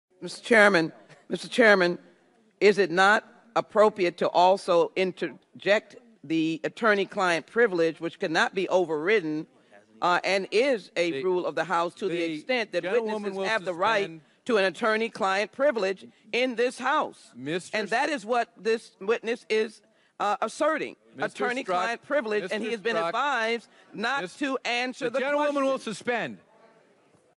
Peter Strzok Testimony Highlights: "THE GENTLEWOMAN WILL SUSPEND!"
Chairman of the House Bob Goodlatte tells Sheila Jackson Lee to STFU, Congress style.

Trying this on your wife or significant other is NOT recommended, but if you do it, please record and upload it!